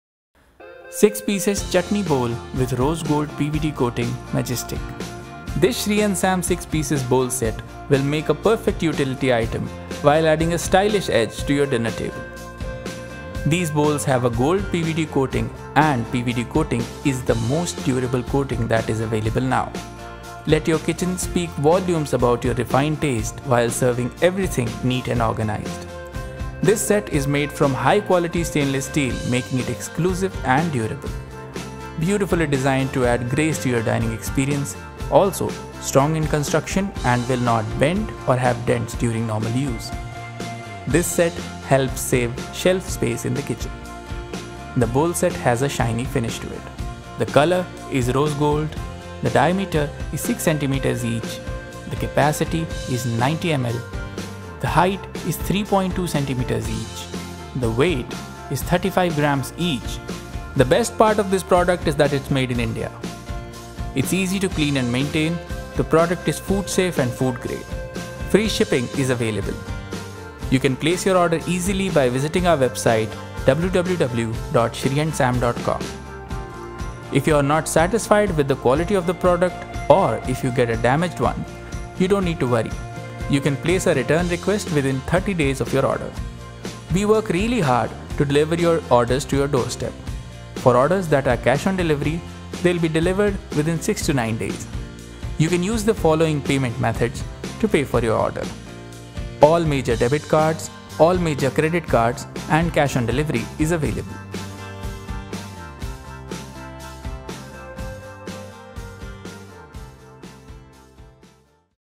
Stainless Steel 6 PCS Chutney Bowl with Rose Gold PVD Coating Majestic (Feature)
This Shri and Sam bowl set will make a perfect utility item while adding a stylish edge to your dinner table, this set is made from high-quality stainless steel making it exclusive and durable, beautifully designed to add grace to your dining experience. Also strong in construction and will not bend or have dents during normal use. This set helps save shelf space in the kitchen. This bowl set is made from premium quality raw material and it is very easy to clean. The bowl set has a shiny finish to it and it will magnify the look of any dinner setting.